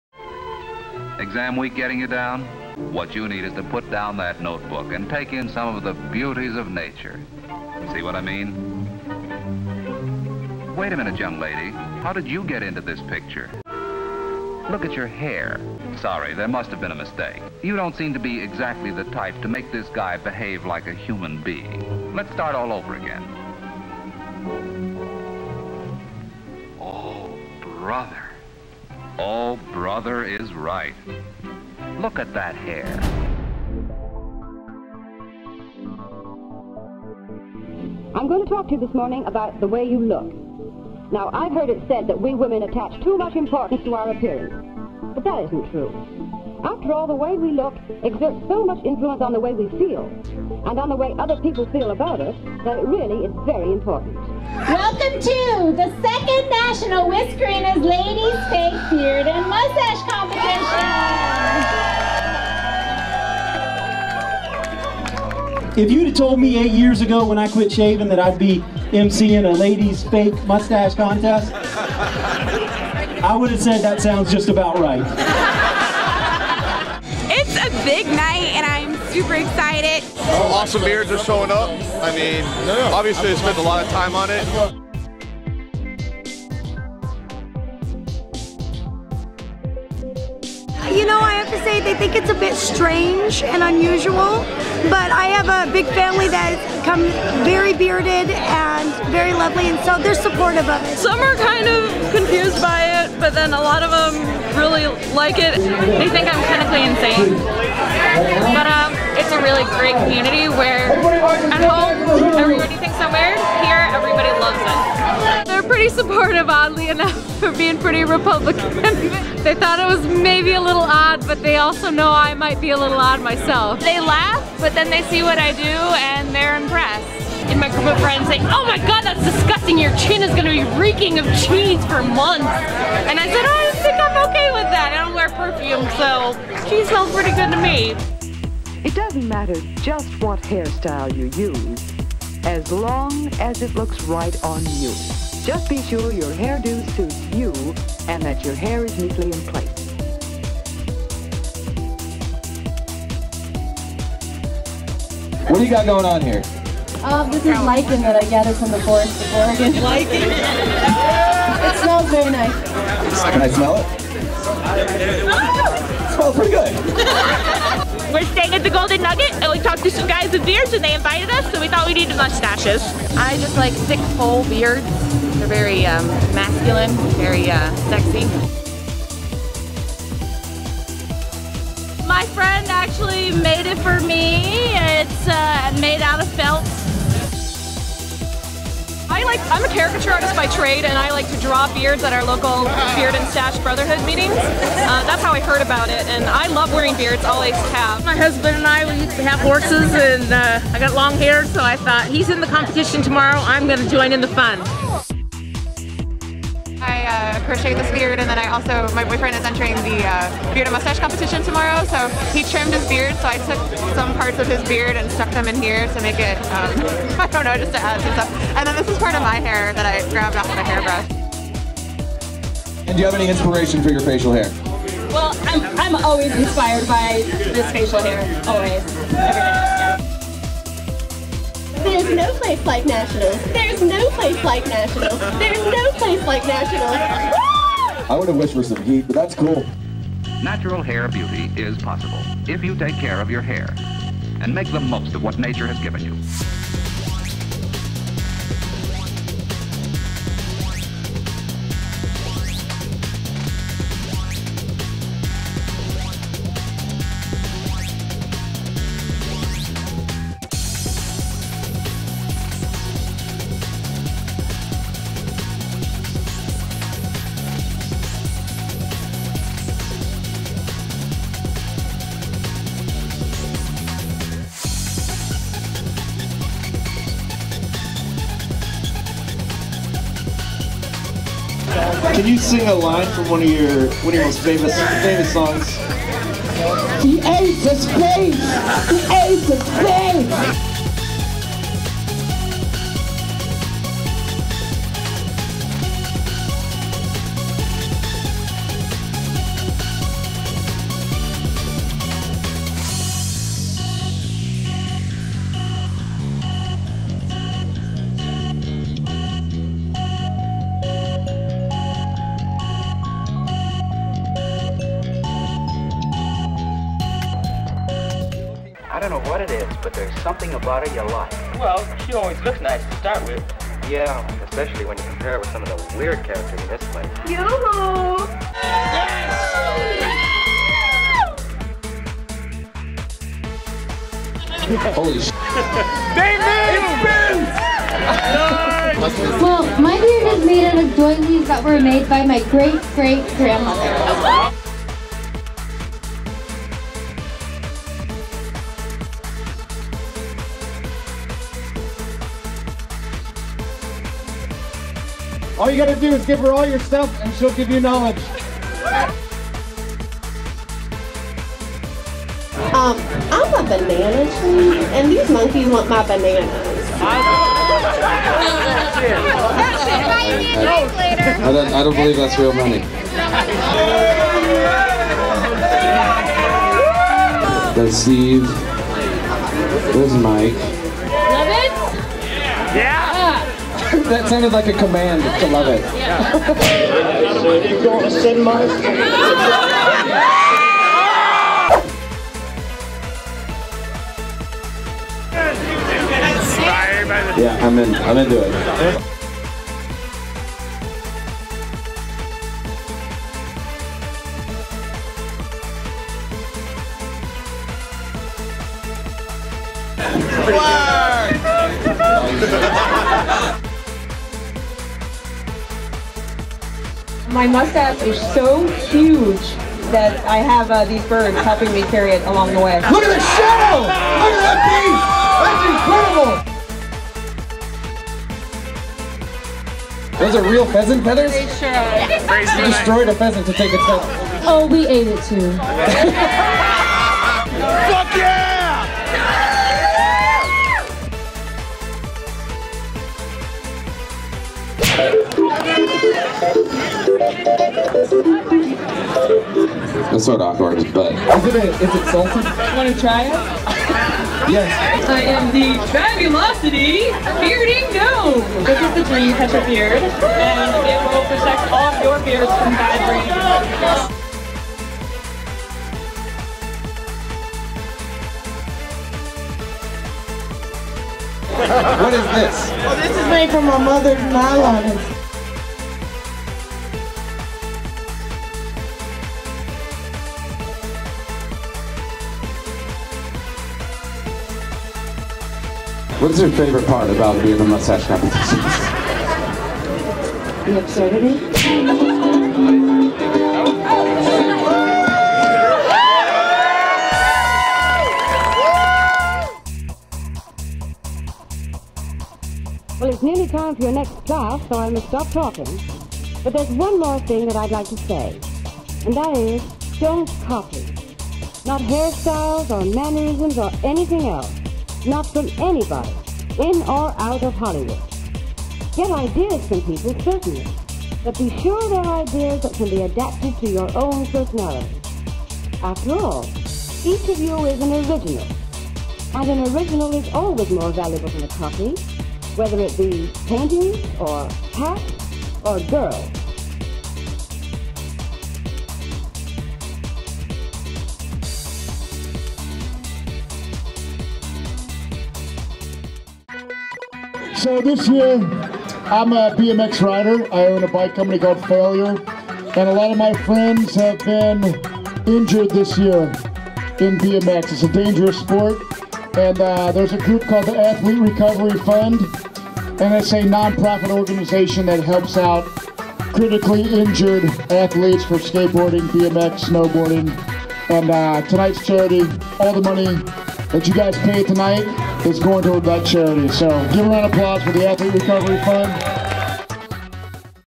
2012 Whiskerinas 2nd National Ladies Fake Beard Comp.
A short video about the Whiskerinas' Second National Ladies Fake Beard & Moustache Competition, held in Las Vegas on November 10, 2012.